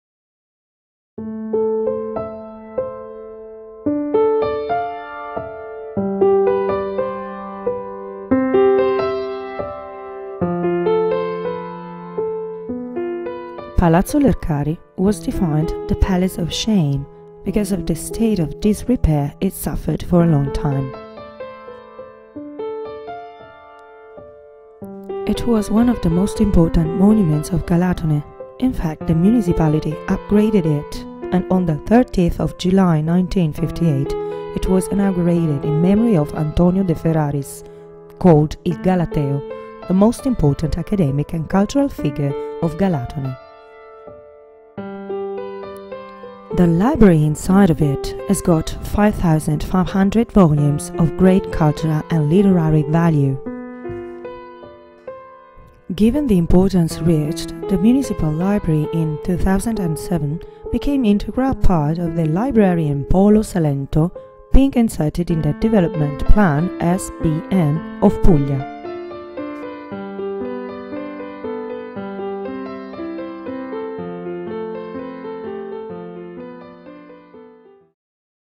City of Galatone - Lercari palace
Palazzo Lercari was defined “the palace of shame” because of the state of disrepair it suffered for a long time.
It was of the most important monuments of Galatone, in fact the municipality upgraded it and on 30th July 1958 it was inaugurated in memory of Antonio De Ferraris, called “Il Galateo”, the most important academic and cultural figure of Galatone.
The palace develops on two floors and the access to the ground floor is allowed by an open-air atrium, formed by 5 rooms.
Only two of them are today occupied by the Historical Archive while the other three by the book heritage formed by the Local Section and the General Section.